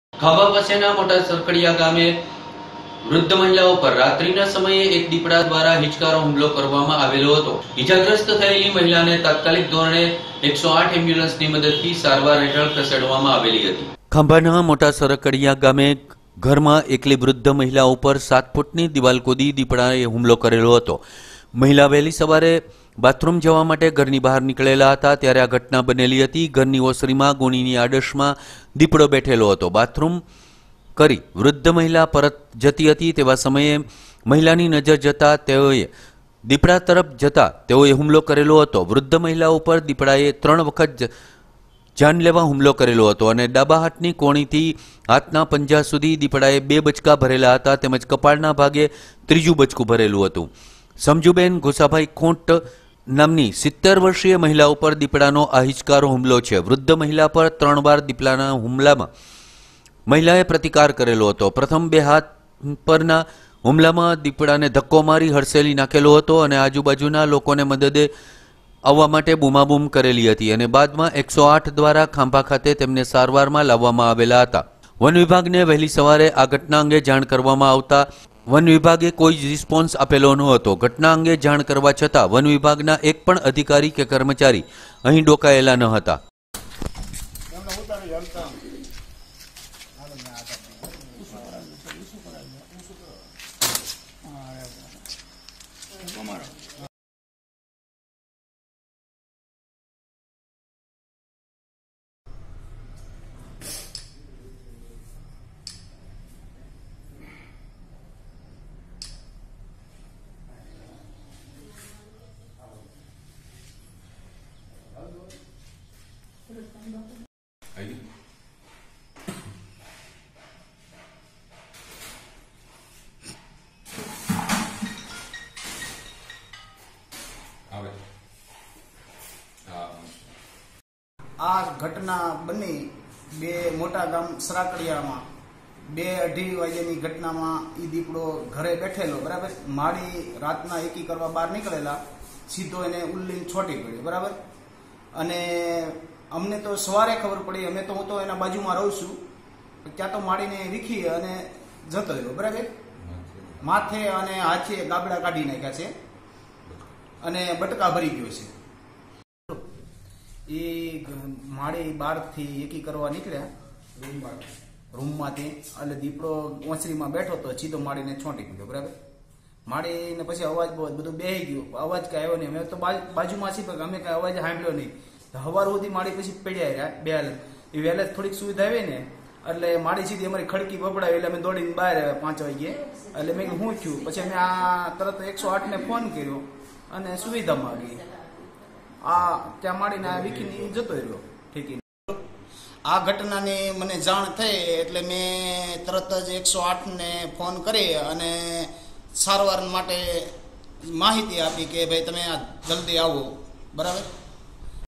ખાંભાના મોટા સરકડીયા ગામે ઘરમાં એકલી વૃધ્ધ મહિલા ઉપર દિપડાએ ૭ ફૂટની દિવાલ કુદી હુમલો કર્યો
સીટી વોચ ચેનલને સબ્સ્ક્રાઇબ કરો અને બેલ આઈકોન ને કલીક કરો રોજ નવી અપડેટ મેળવતા રહો